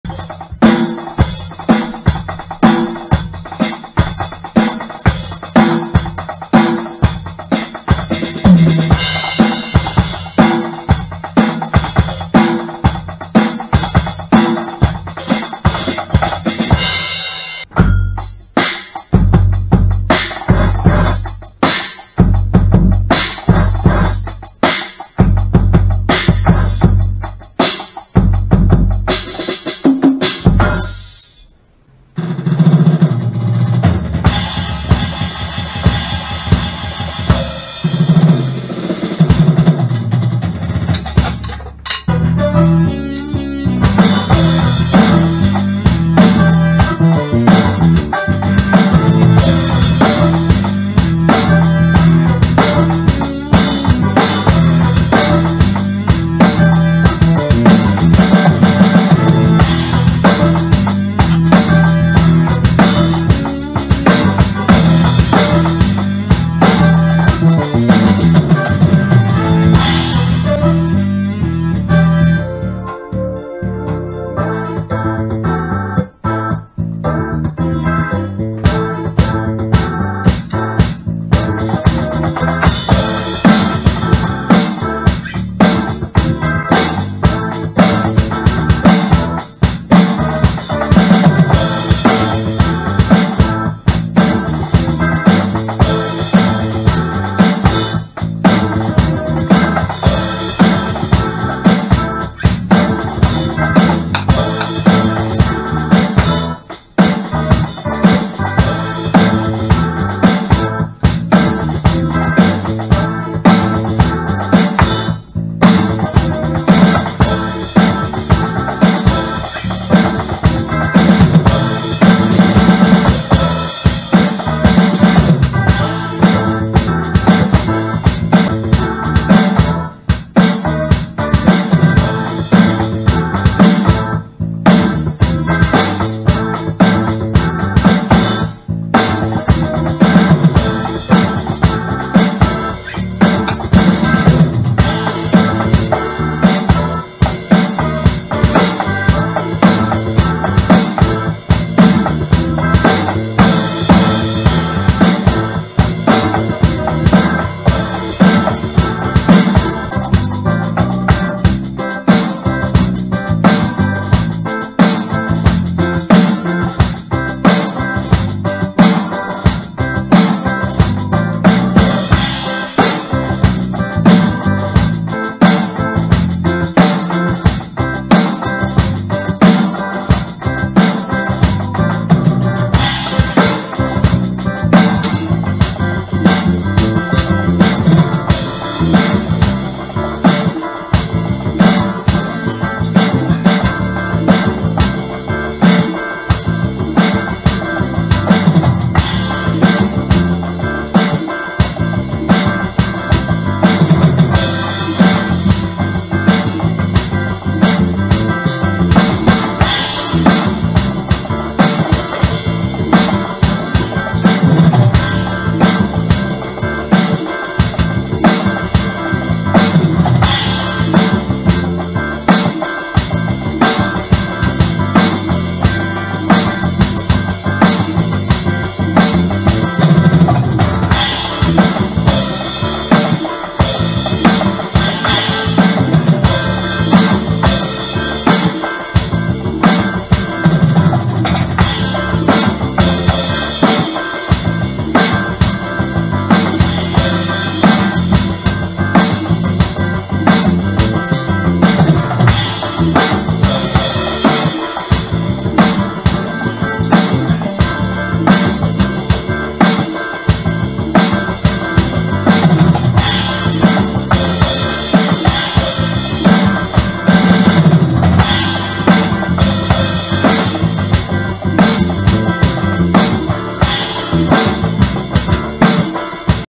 Playing drums again after 12 years off!
Haven’t had a drum set for about 12 years until now. Just picked up this Alesis Command X Mesh Special Edition. Enjoying it so far.